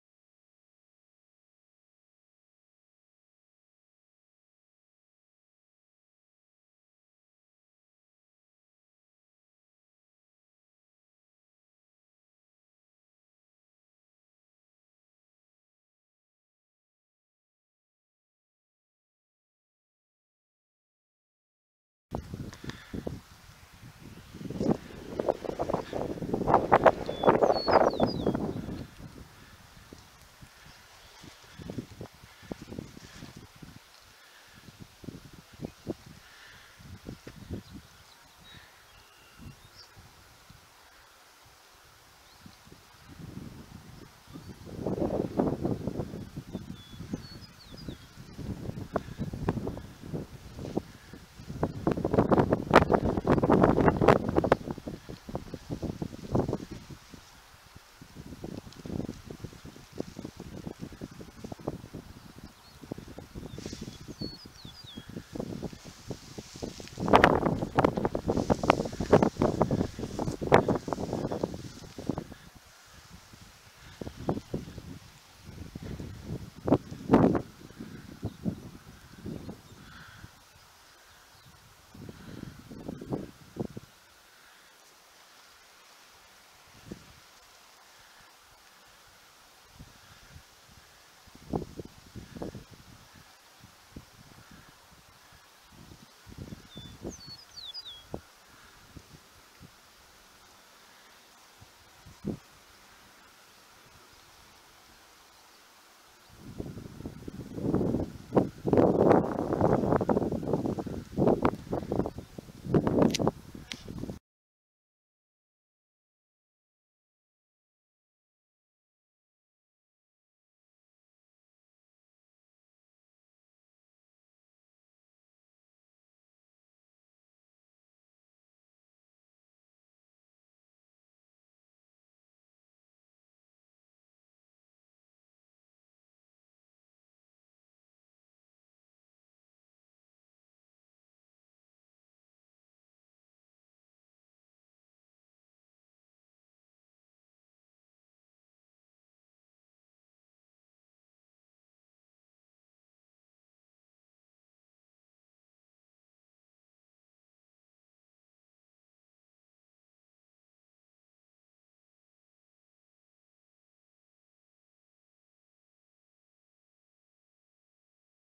Power Foil Traction Kiting
Peter Lynn Skim 3.4M 3-line water relaunchable kite.Video location Roger Birdsall Memorial Park.